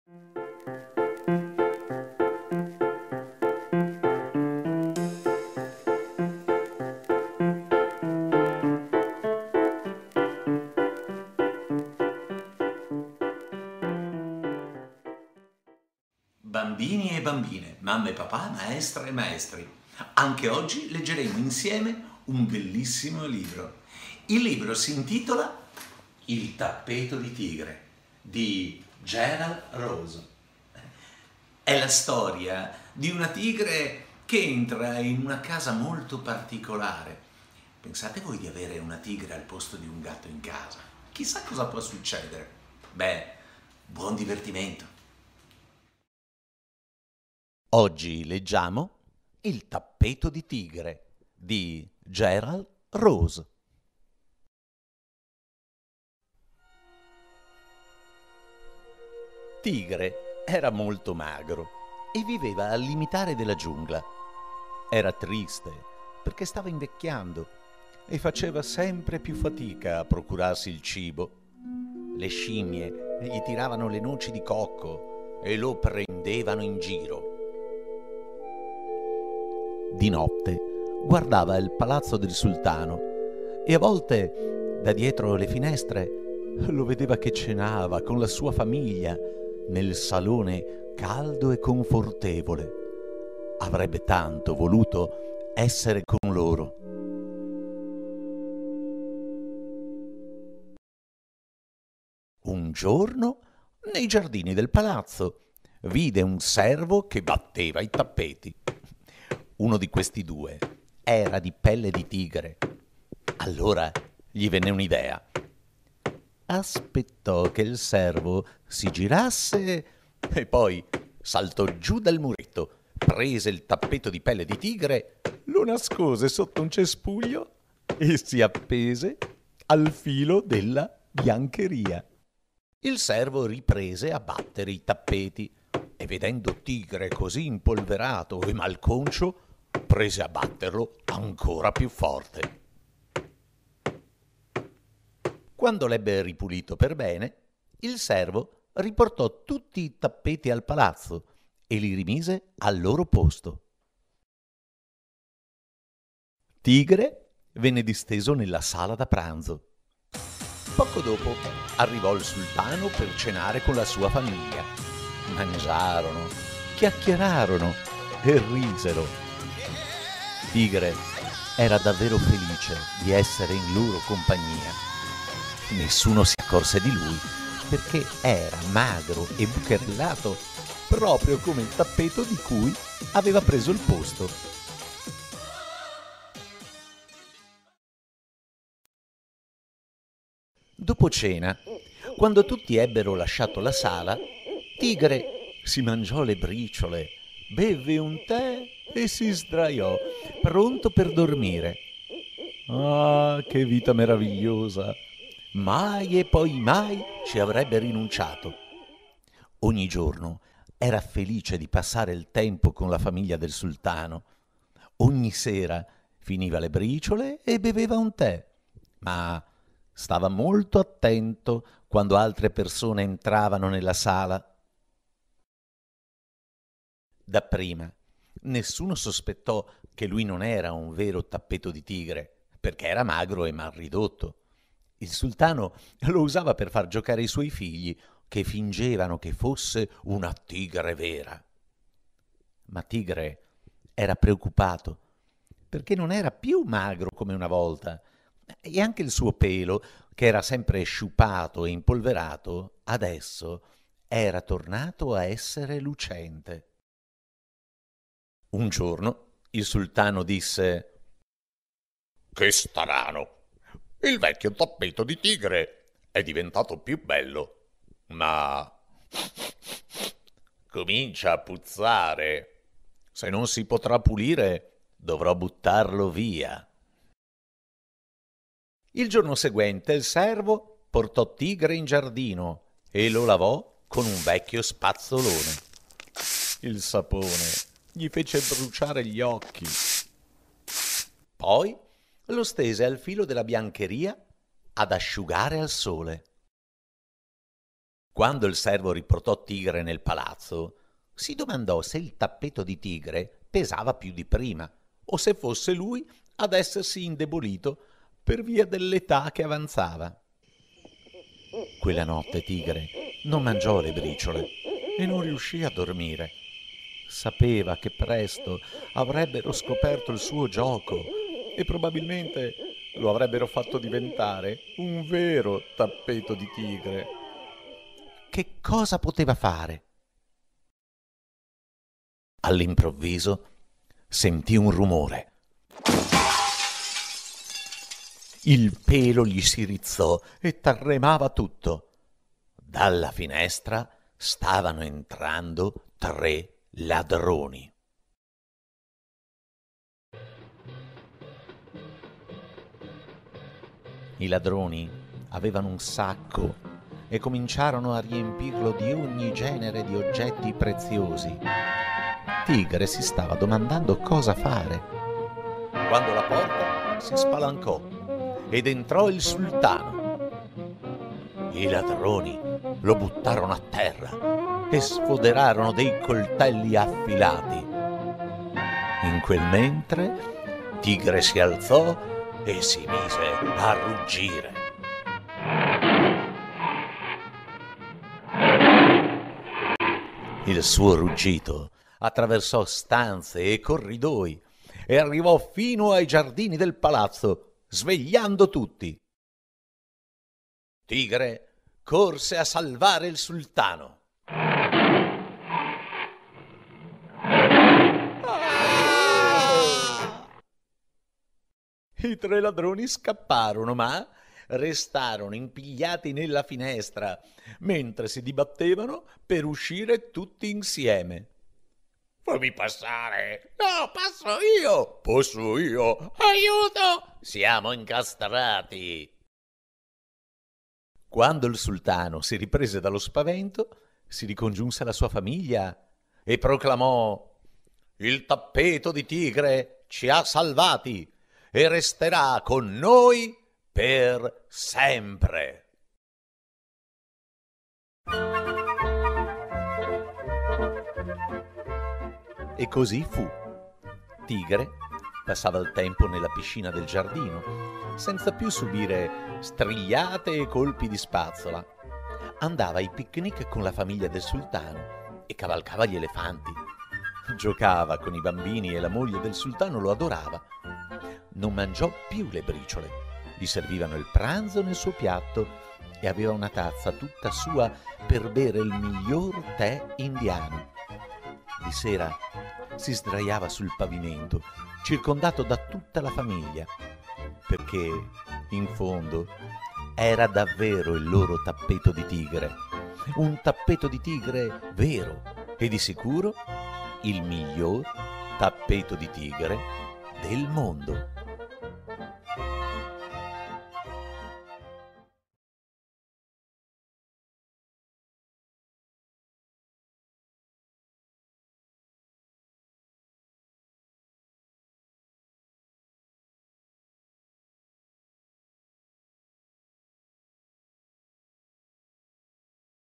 Il tappeto di tigre
Ortoteatro presenta
IL TAPPETO DI TIGRE di Gerald Rose
Nord-Sud Edizioni.
Una lettura di Fabio Scaramucci.
Progetto Educare & Co. della FISM Federazione Italiana Scuole Materne Pordenone, Con i Bambini Fondo per il Contrasto della Povertà Educativa Minorile e Ortoteatro